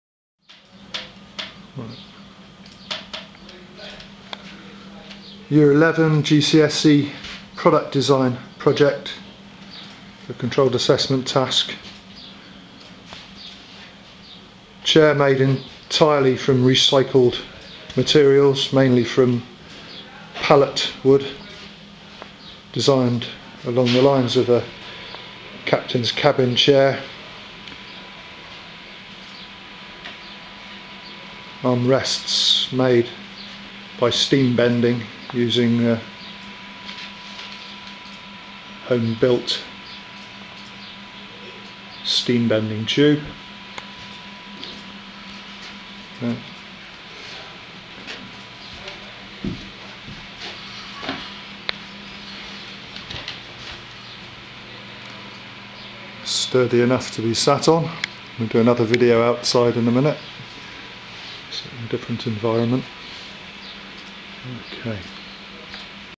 Killian Sunderland GCSE AQA Product Design Chair
Made from reclaimed pallet wood, includes steam bent arm rest!